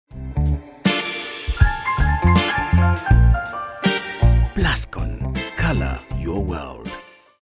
Plascon's WeatherGuard Extreme
Plascon's WeatherGuard Extreme is a uniquely formulated coating based on an acrylic copolymer emulsion to provide the ultimate protection to all new and old buildings